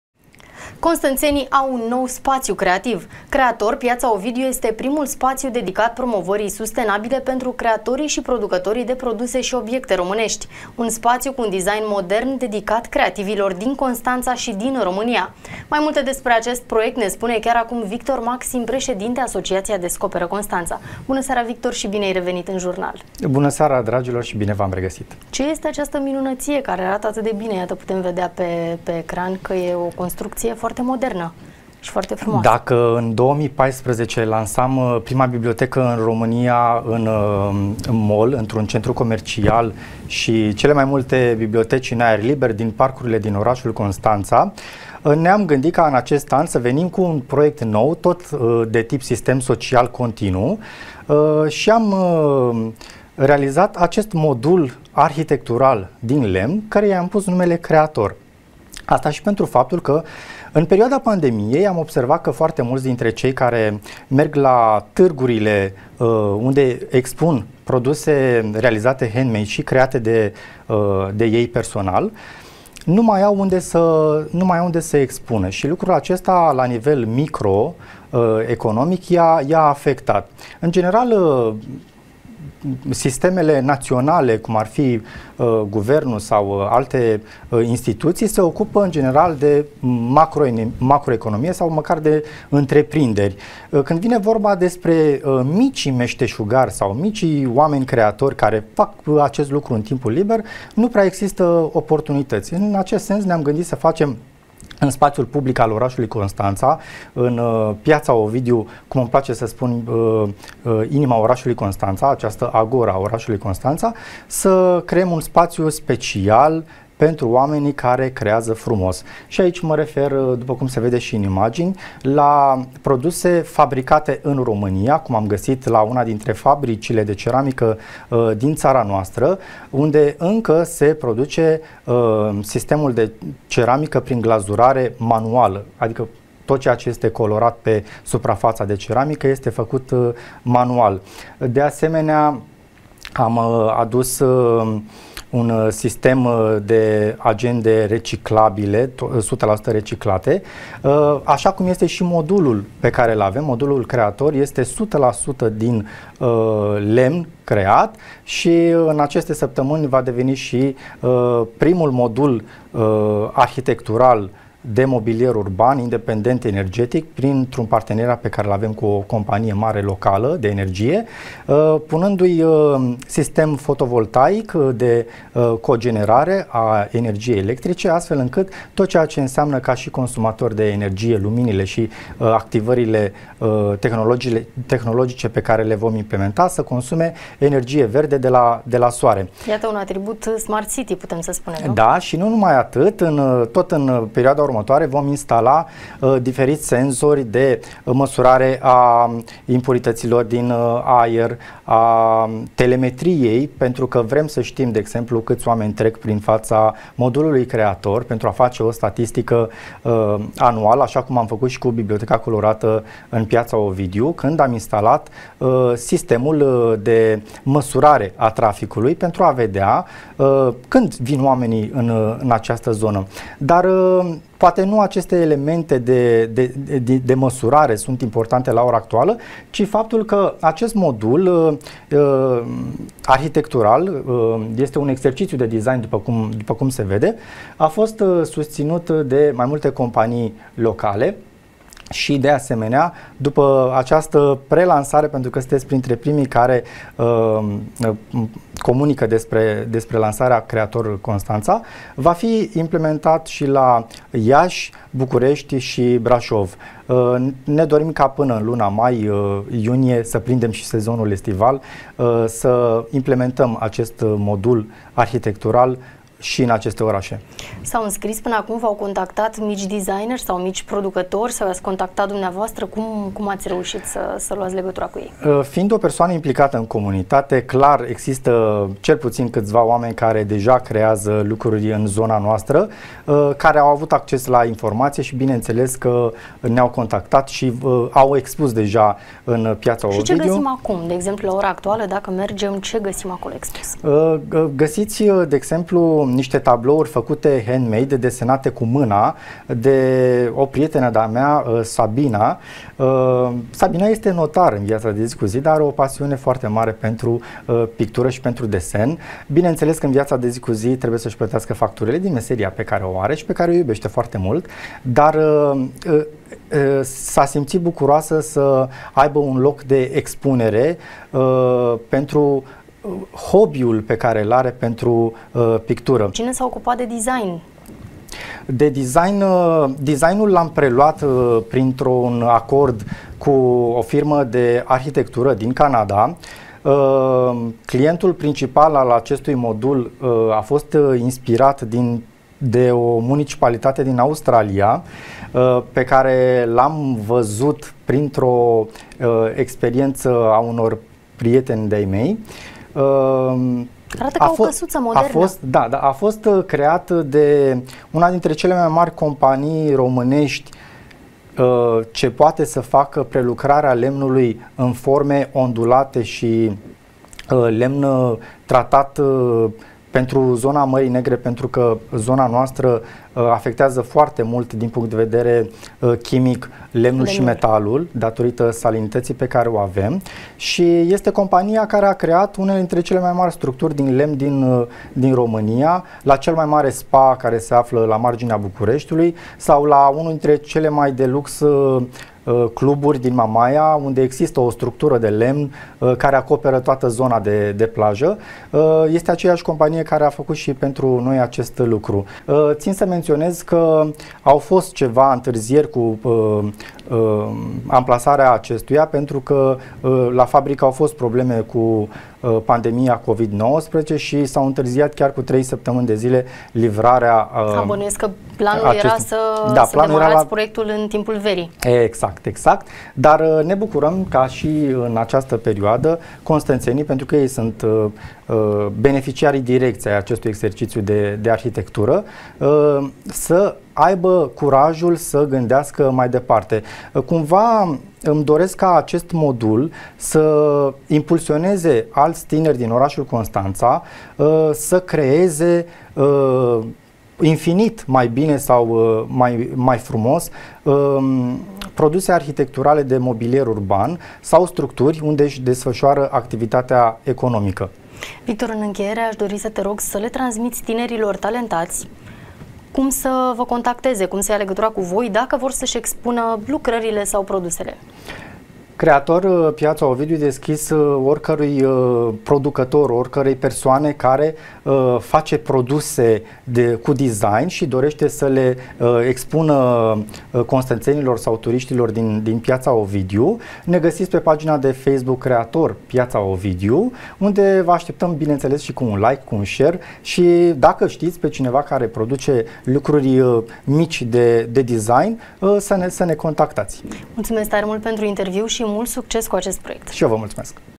Constănțenii au un nou spațiu creativ!
Creator Piata Ovidiu este primul spatiu dedicat promovãrii sustenabile pentru creatorii si producãtorii de produse si obiecte românesti. Un spatiu cu un design modern dedicat creativilor din Constanta si din România. Mai multe despre acest proiect ne spune chiar acum Victor Maxim - președinte Asociația Descoperă Constanța.